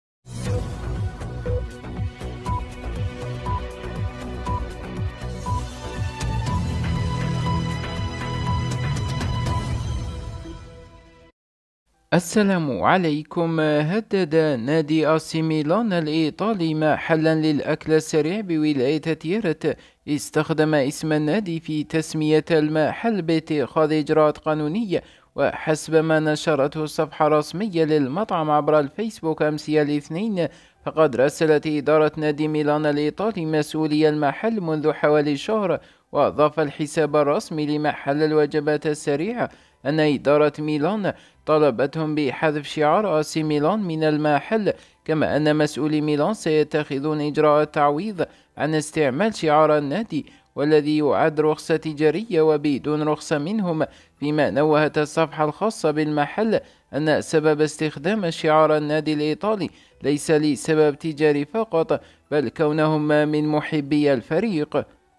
عاجل 🔴 نادي اسي ميلان الايطالي يراسل صاحب محل للأكل السريع في ولاية تيارت ويطالبه بتغيير اسم محله...
هدد نادي آسي ميلان الإيطالي، محلا للأكل السريع بولاية تيارت، إستخدم إسم النادي في تسمية المحل باتخاذ إجراءات قانونية.

وحسب ما نشرته الصفحة الرسمية للمطعم عبر الفايسبوك، أمس الإثنين، Pizzeria Milano، فقد راسلت إدارة نادي ميلان الإيطالي، مسؤولي المحل، منذ حوالي شهر.

وأضاف الحساب الرسمي لمحل الوجبات السريعة، أن إدارة ميلان طالبتهم بحذف شعار آسي ميلان من المحل.

كما أن مسؤولي ميلان، سيتخذون إجراءات تعويض عن استعمال شعار النادي، الذي يعد رخصة تجارية، وبدون رخصة منهم.

فيما نوهت الصفحة الخاصة بالمحل، أن سبب إستخدام شعار النادي الإيطالي، ليس لسبب تجاري فقط بل كونهم  محبي الفريق.